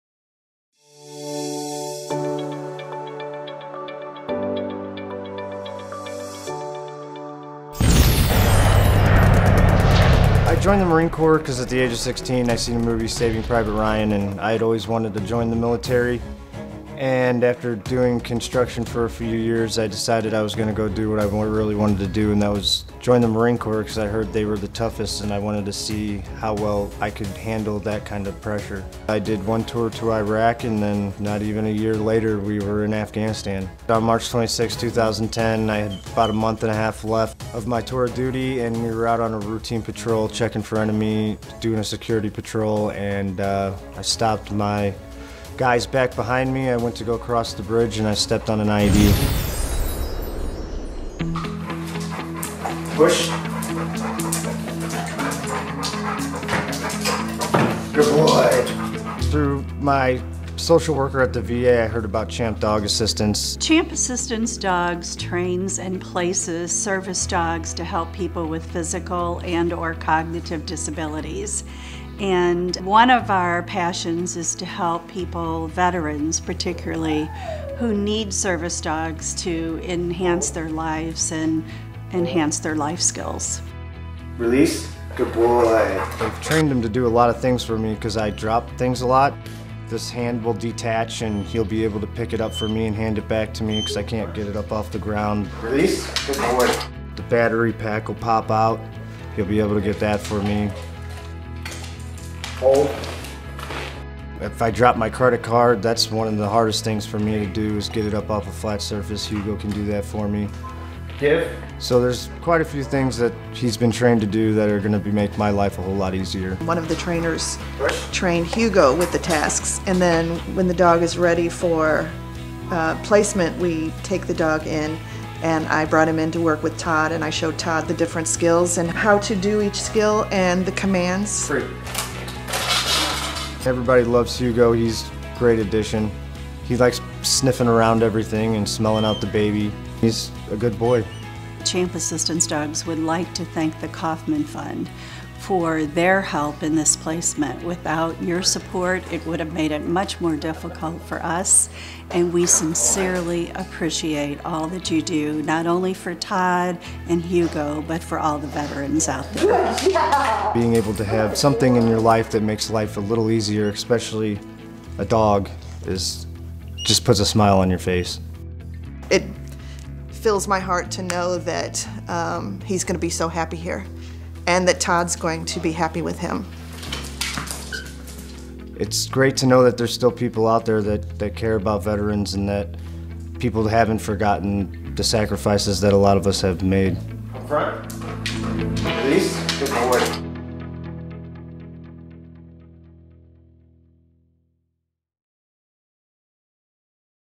Todd & Hugo - CHAMP Assistance Dogs
This video tells the story of a veteran and his assistance dog.